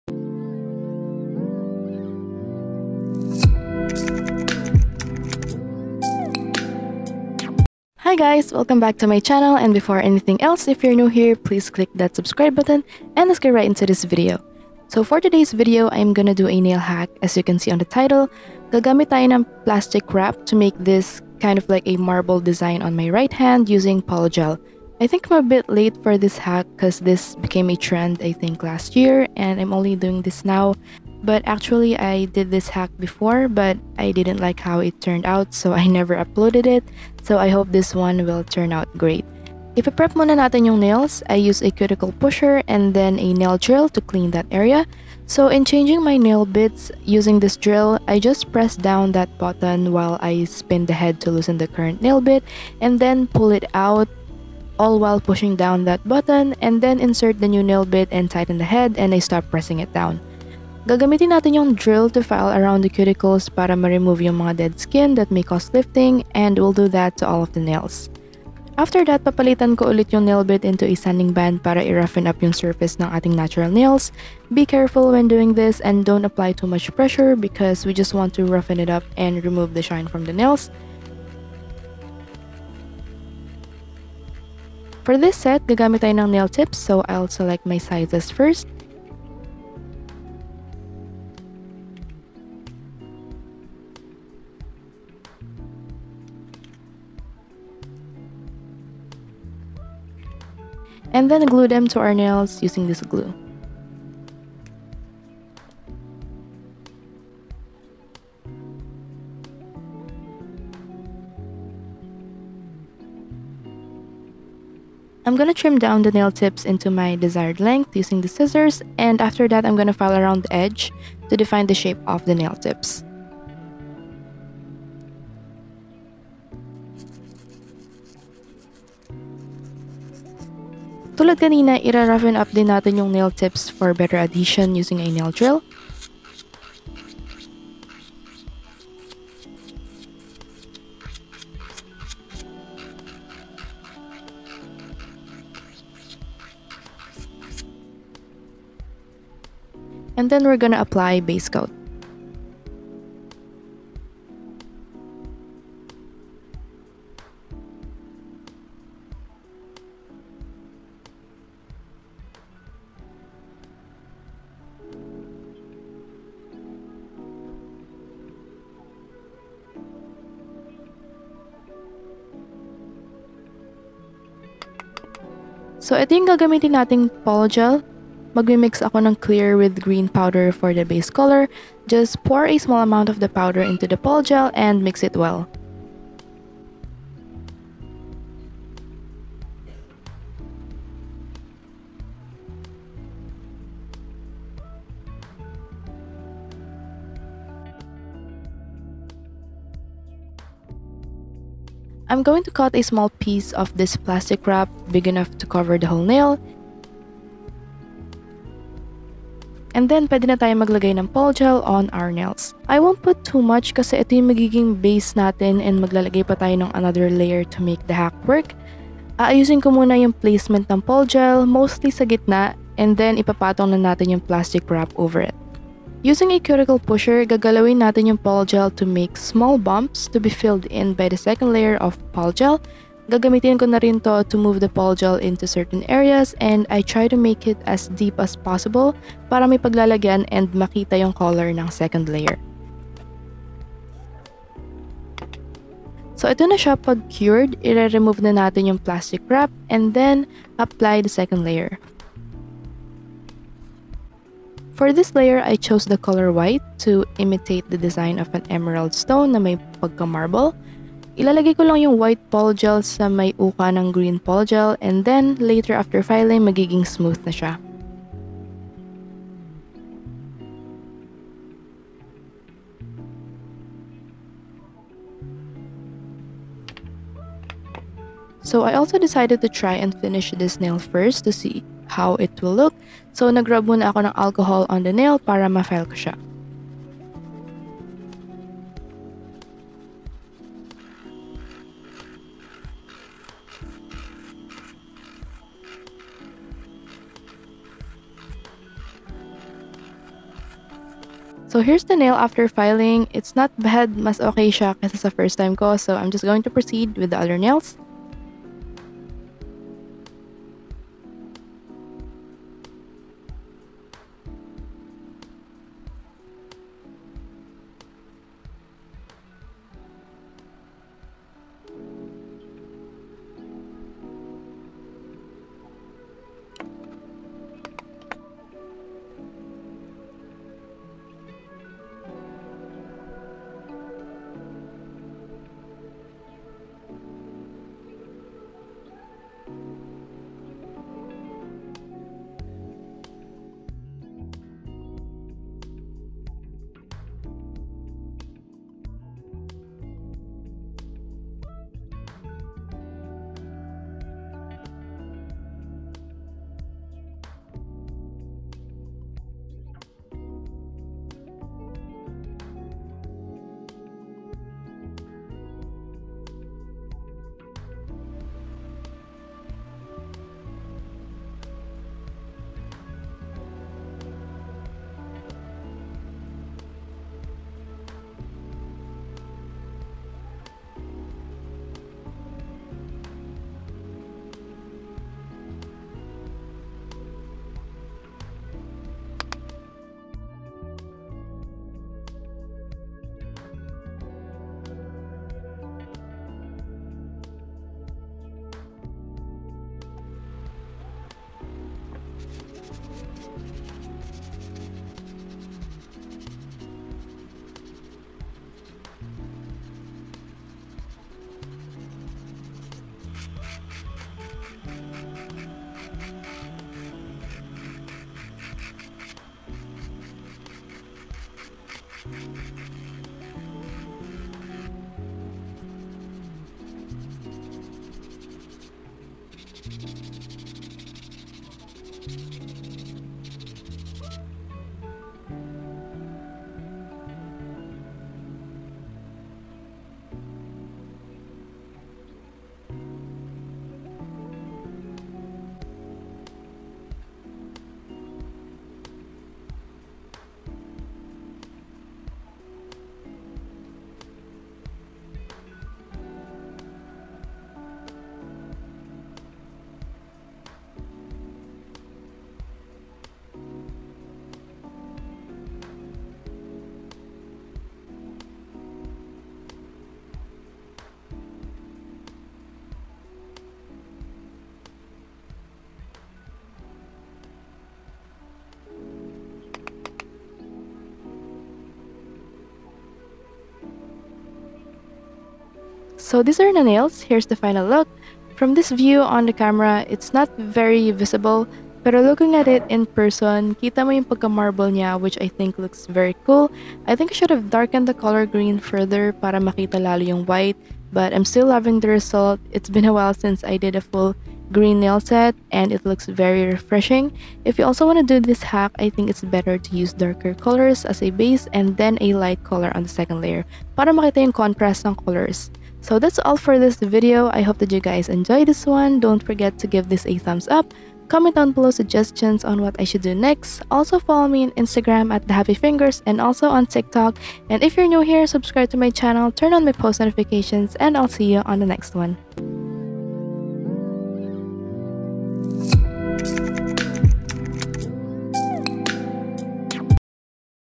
PLASTIC WRAP NAILS USING POLYGEL | Emerald-Inspired Nail Design 💚 Polygel Nails Tutorial
[ENG SUB] Hi guys! In this video, I tried the plastic wrap nail hack to make a marbled look and I made an Emerald-Inspired Nail design on my right hand! 💚

*some nail/nail art tools are not purchased online so i was not able to provide the links for those. i purchased most of them from a nearby store, mostly from retail stores.